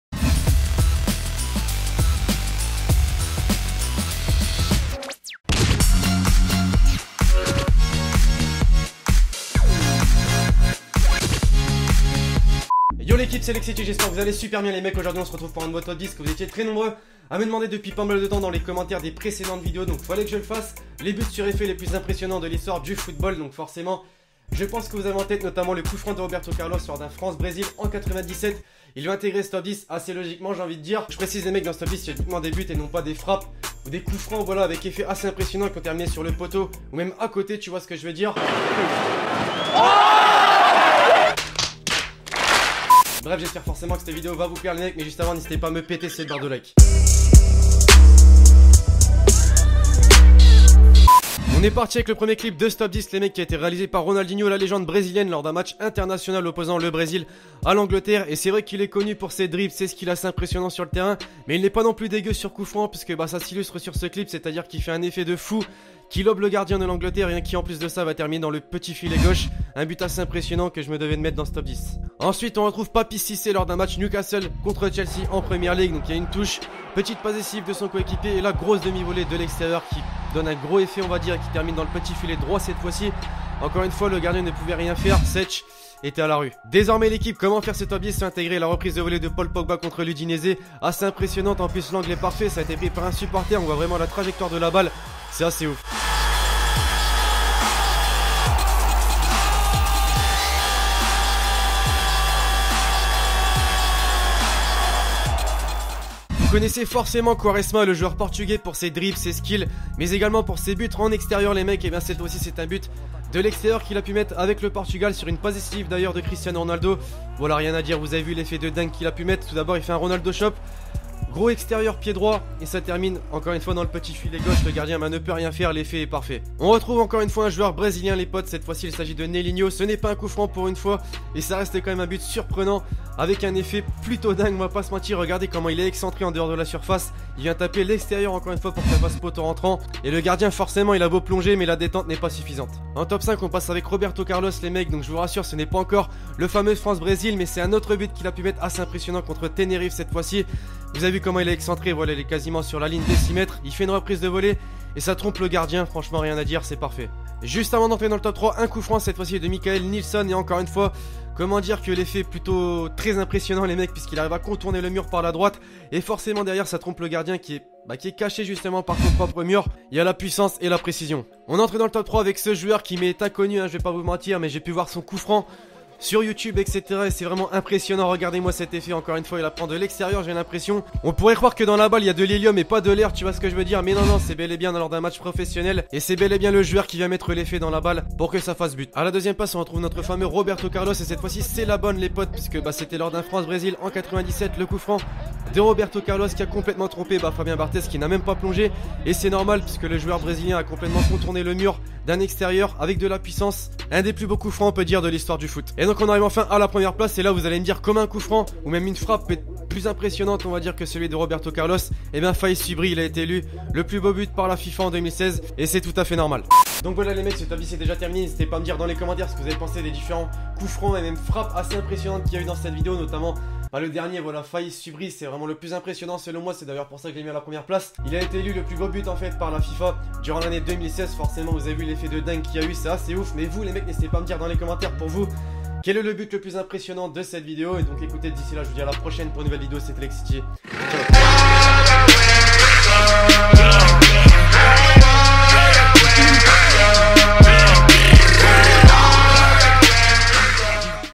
LES BUTS AVEC DES EFFETS DE FOU DANS LE FOOT !
LES BUTS AVEC DES EFFETS IMPRESSIONNANT DANS LE FOOT !!! TOP 10 CURVED GOALS IN FOOTBALL HISTORY !! FAIZ SUBRI, ROBERTO CARLOS, ETC.. ! - FIFA 17 ULTIMATE TEAM !

Salut à tous ! On se retrouve aujourd'hui pour une nouvelle vidéo, un nouveau concept sur la chaîne : Regrouper les exploits par thème des joueurs de foot IRL, donc aujourd'hui c'est consacré aux buts avec effets les plus impressionnants du foot ! (TOP 10)
Dans cette vidéo il y a les buts de :
- Faiz Subri Puskas Award 2016 free kick
- Roberto Carlos coup franc France Brésil 1997
- Geraldo Pereira curved free kick
- Mikael Nilsson free kick
- Roberto Carlos volley goal vs Tenerife
- Nelinho curve
- Ricardo Quaresma exterieur du pied
- Pogba volley vs Udinese
- Papiss Cissé goal vs Chelsea curved volley
- Ronaldinho free kick Brazil

(FR/Français)

♫ CRÉDITS AUDIO ♫
• I have got all the licenses of the musics that I use in the video.